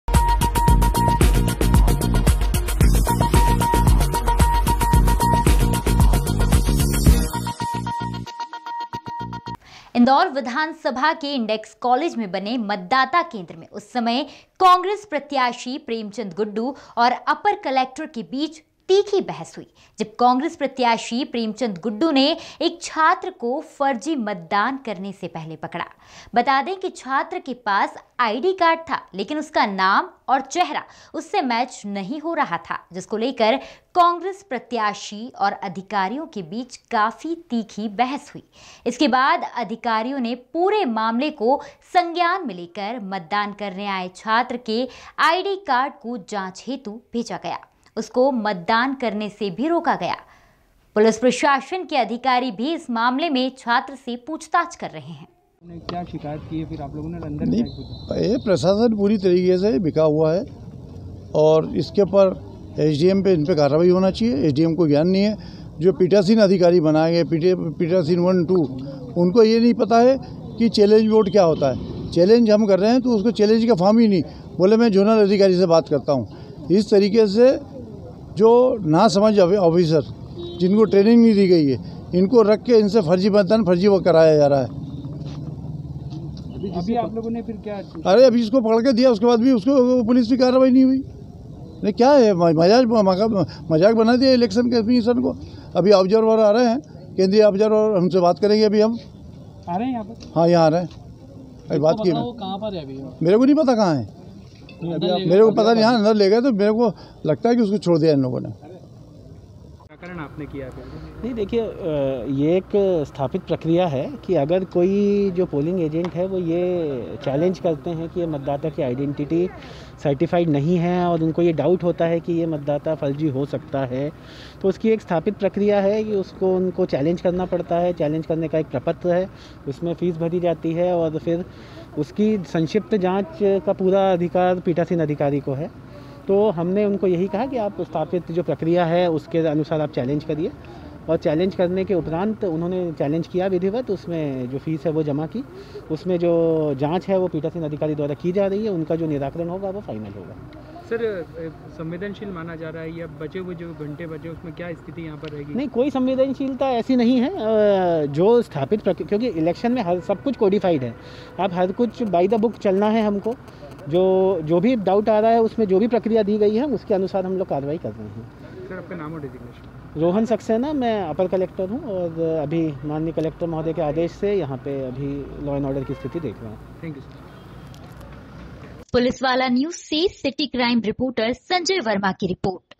इंदौर - फर्जी मतदाता पकड़ाया
इंदौर विधानसभा के इंडेक्स कॉलेज में बने मतदाता केंद्र में उस समय कांग्रेस प्रत्याशी प्रेमचंद गुड्डू और अपर कलेक्टर के बीच तीखी बहस हुई जब कांग्रेश प्रत्याशी प्रेमचंद गुड्डू ने एक छात्र को फर्जी मतदान करने से पहले पकड़ा छात्र के पास आईडी कार्ड था लेकिन उसका नाम और चेहरा उस से मैच नहीं हो रहा था जिसको लेकर कांग्रेस प्रत्याशी और अधिकारियों के बीच काफी तीखी बहस हुई इसके बाद अधिकारियों ने पूरे मामले को संज्ञान में लेकर मतदान करने आए छात्र के आईडी कार्ड को जांच हेतु भेजा गया उसको मतदान से रोका गया  पुलिस प्रशासन के अधिकारी भी इस मामले में पूछताछ कर रहे हैं